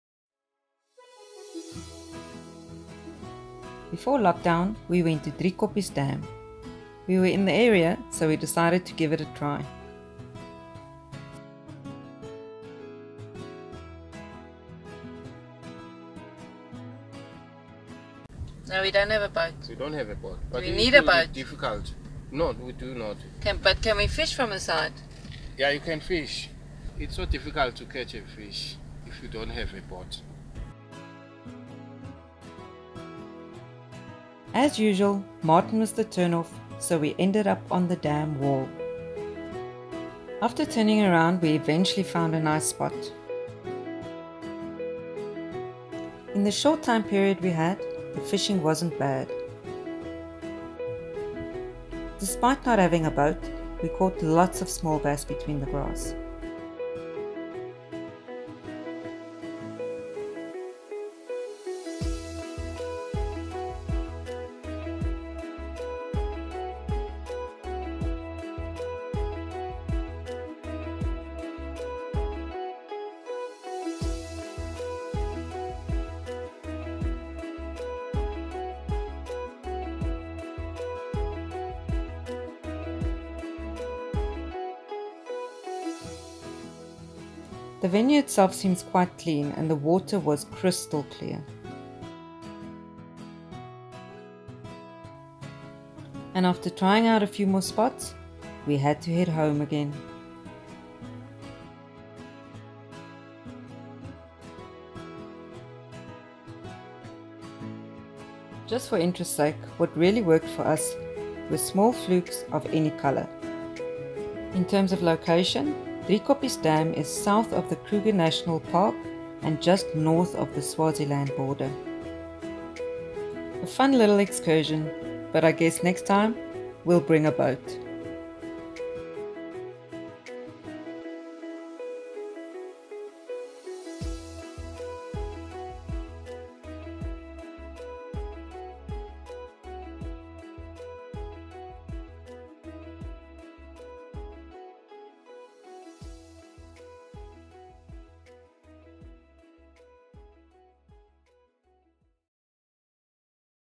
Bass fishing at Driekoppies Dam, South Africa (Mar 2020)
Before lockdown we had the opportunity to visit Driekoppies Dam near Malelane in Mpumalanga, South Africa.

We didn't expect much and the fact that we did not have a boat really hampered our efforts but we still had a fun afternoon there. We caught lots of small bass in the shallows among the grass. No big ones but lots of fun still.

If we ever return we'll make sure to have a boat :-)

Songs:
1.